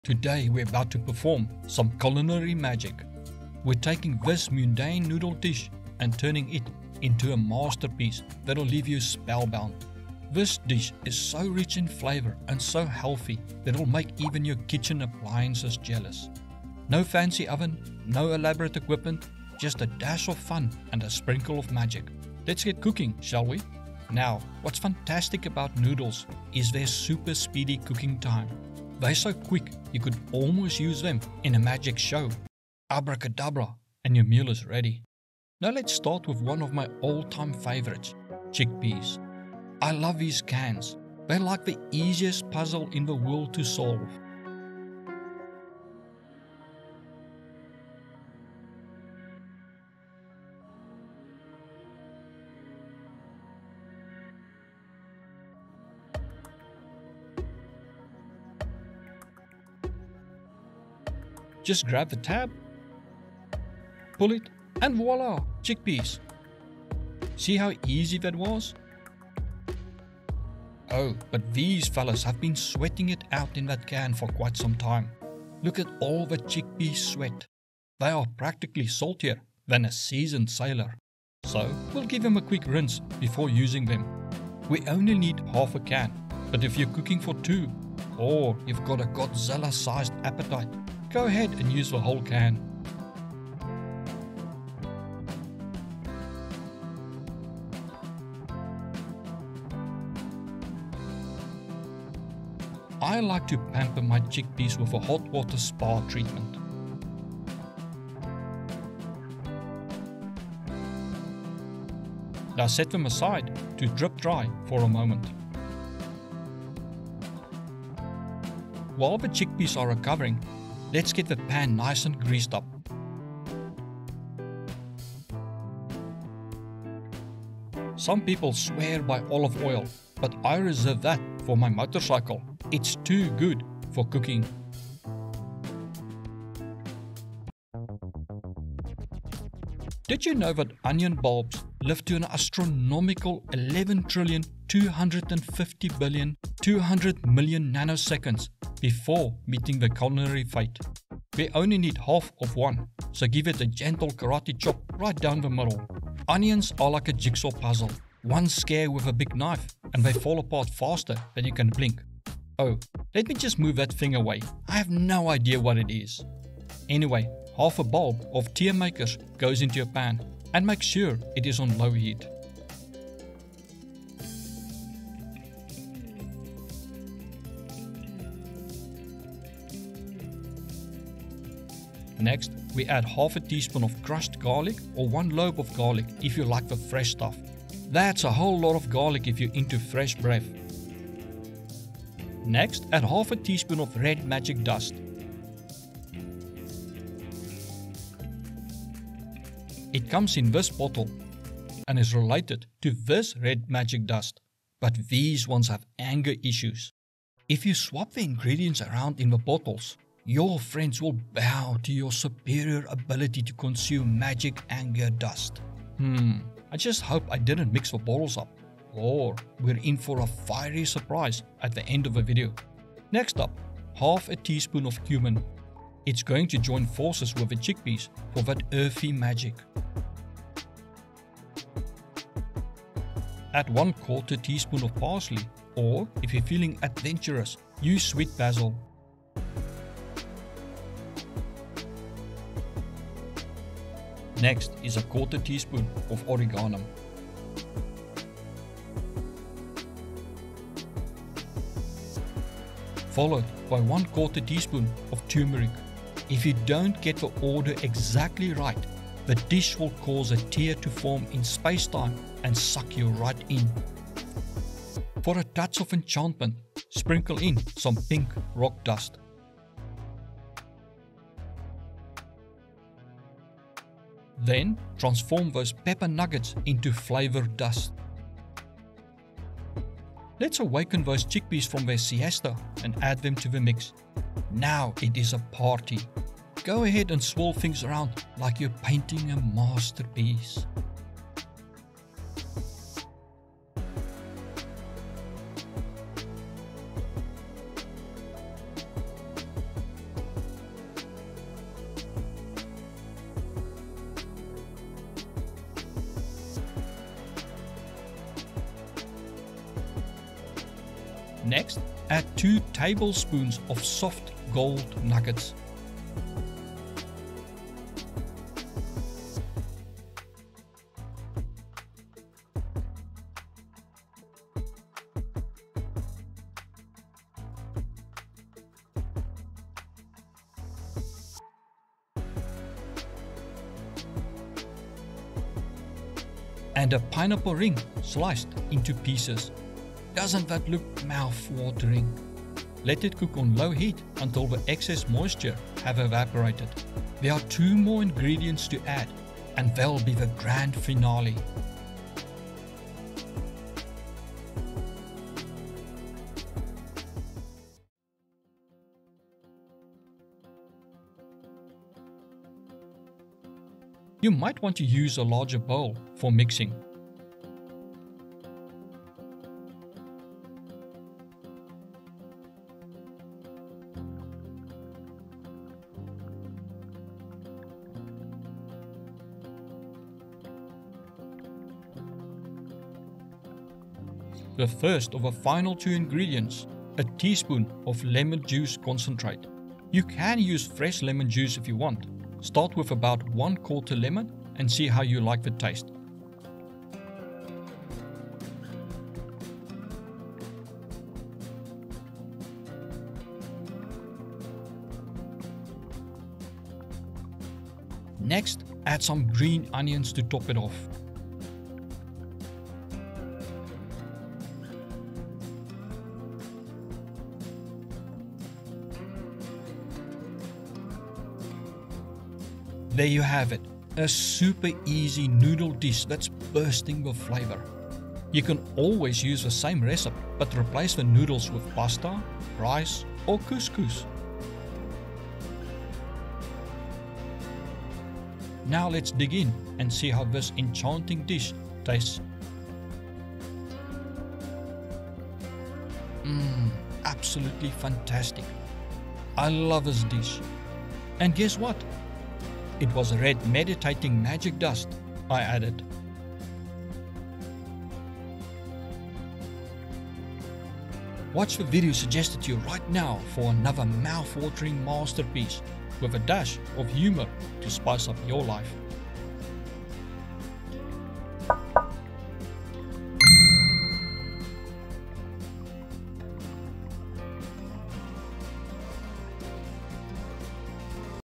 The Best Noodles You Will Ever Have
Prepare to be amazed as we take ordinary noodles and turn them into a culinary masterpiece, complete with a sprinkle of laughter and a dash of magic! In this video, we're whipping up a quick and flavorful Chickpea Noodle dish that will leave you craving for more.

Whether you're a kitchen novice or a seasoned pro, you're in for a treat. We promise this dish will have you laughing and salivating at the same time! It's the perfect solution for those busy weeknights when you're craving a flavorful meal in a hurry.

Join us in this fun-filled culinary journey and discover how to make the most out of your noodles.